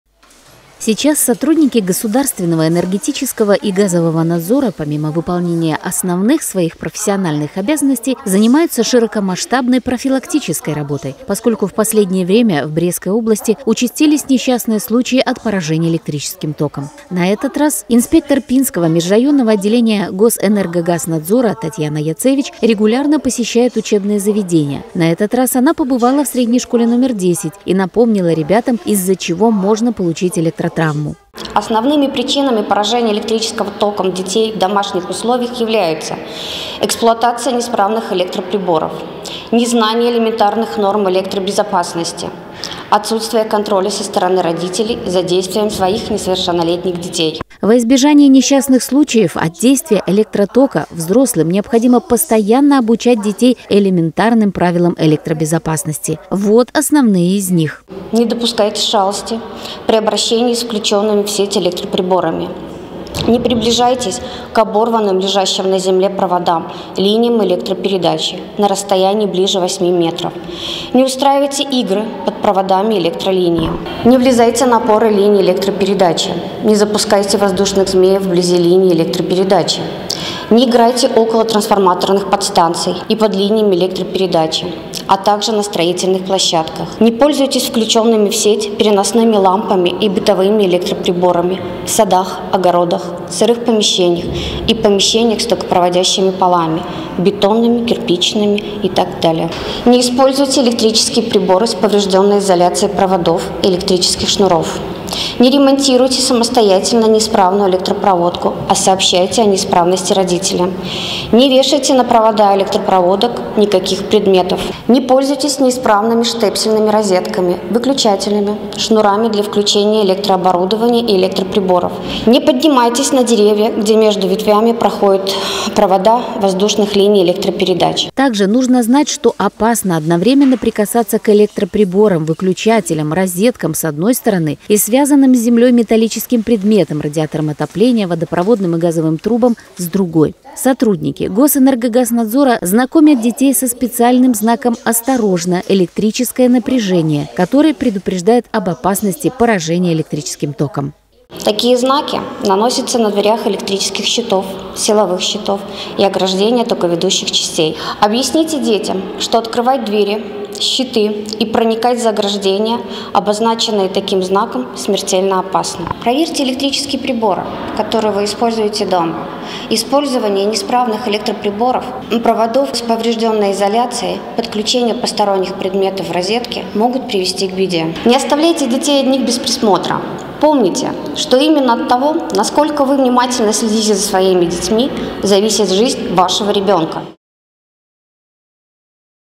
Пинскэнергогазнадзор: профилактика электротравм в быту
То и дело в сводке несчастных случаев фигурируют факты гибели детей от поражения электрическим током. Такие случаи участились в последнее время. Малыши в дошкольном возрасте очень любознательны и отсутствие жизненного опыта может привести к электротравмам. В основном несчастные случаи происходят из-за недосмотра родителей. Потому в нашем следующем материале с участием инспектора Госэнергогазнадзора мы ещё раз напомним правила электробезопасности, которые смогут сохранить здоровье и жизнь вашему ребенку.